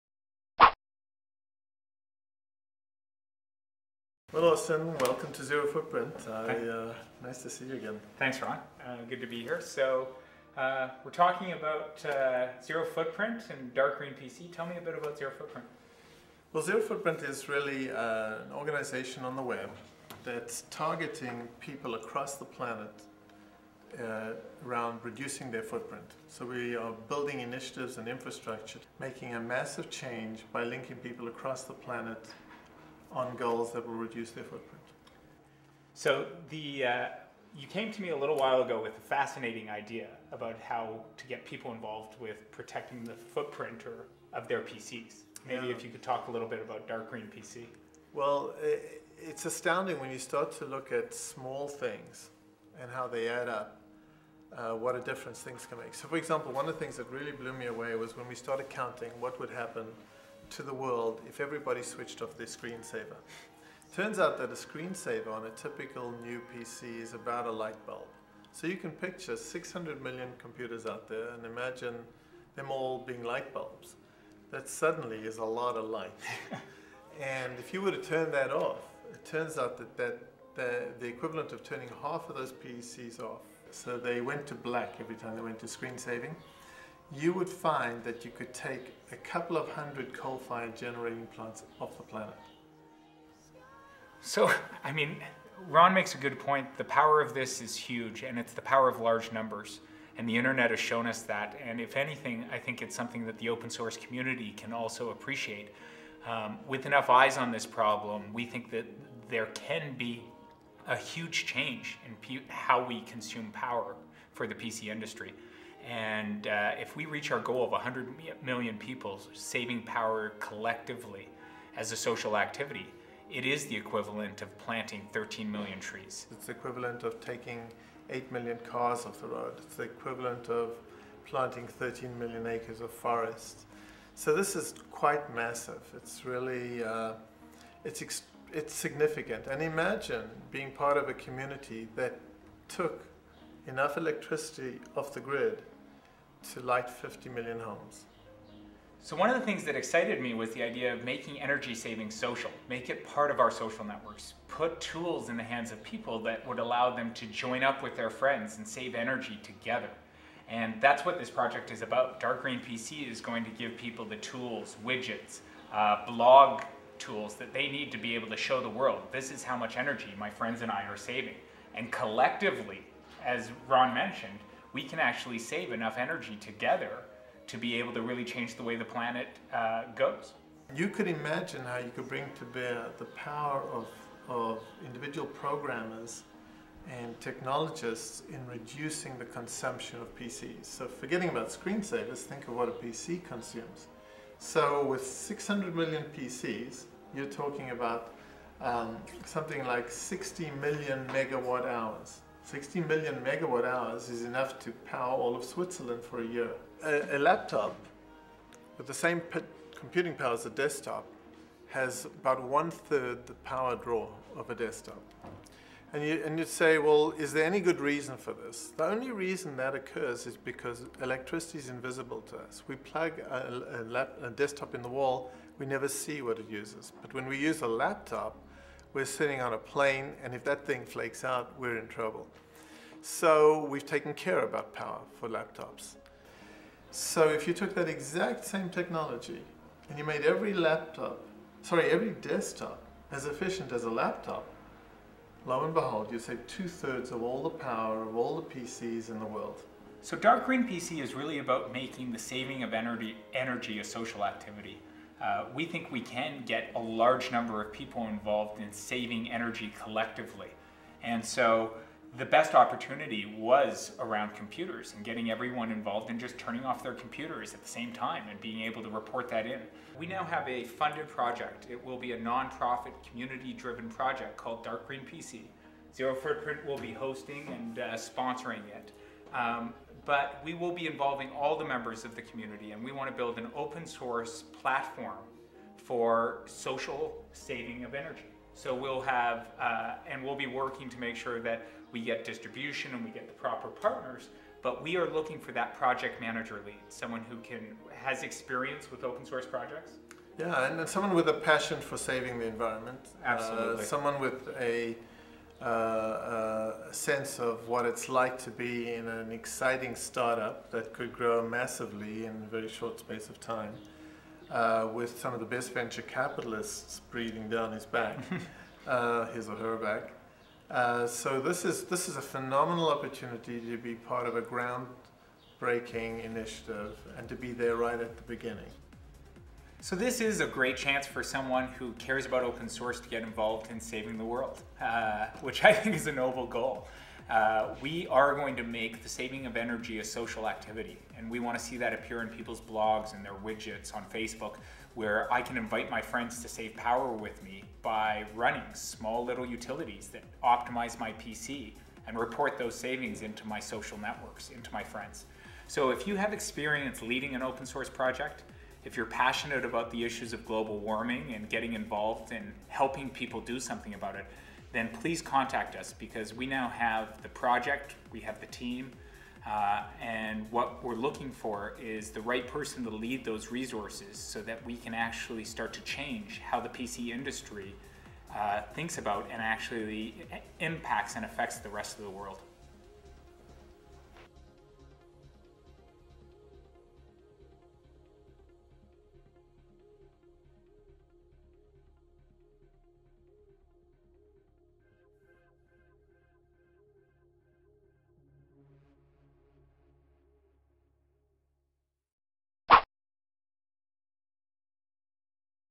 DarkGreenPC: Open Source Project Leader
DarkGreenPC is a non-profit, community organized open source project inspired by Seti@Home and Distributed.net where we are going enable people to optimize the power usage of their computers collectively and turn energy saving into a social activity.

We are now looking to hire an open source product leader.